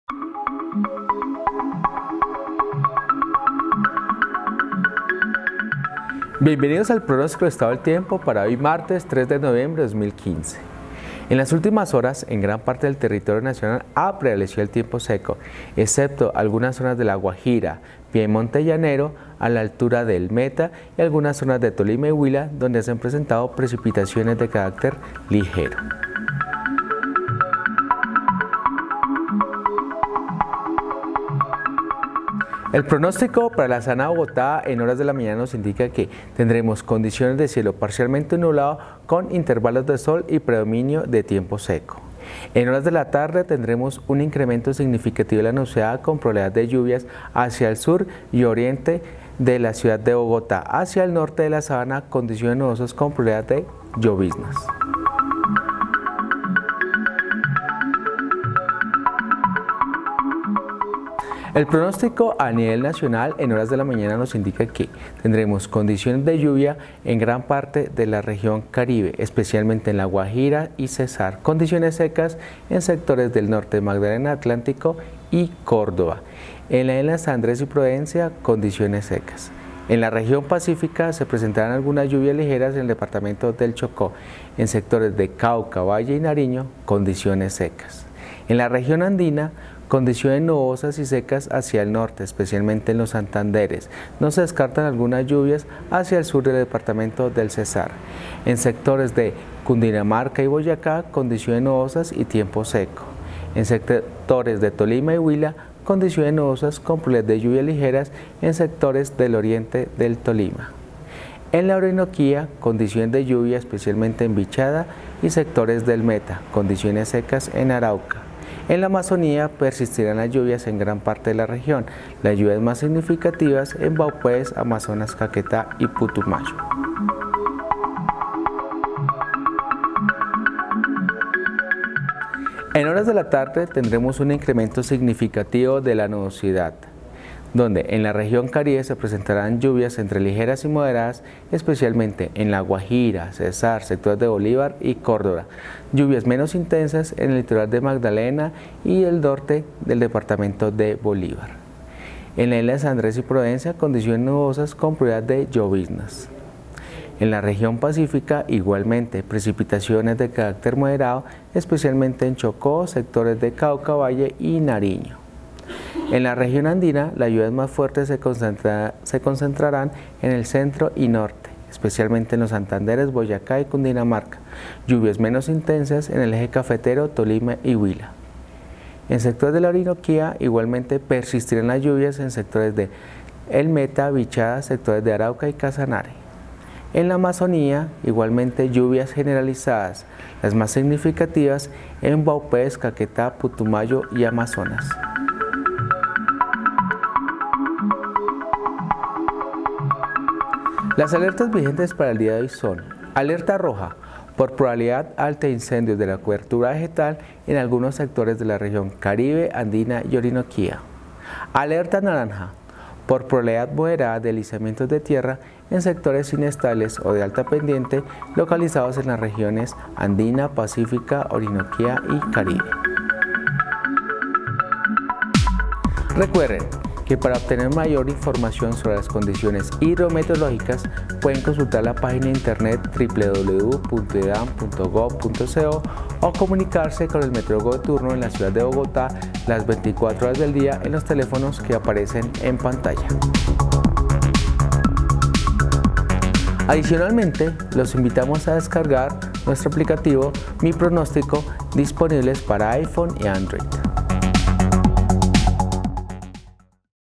Pronóstico del Tiempo - Martes 03 de Noviembre de 2015.
Pronóstico del tiempo virtual para el Martes 03 de Noviembre de 2015, anunciando condiciones Hidrometeorológicas en Colombia.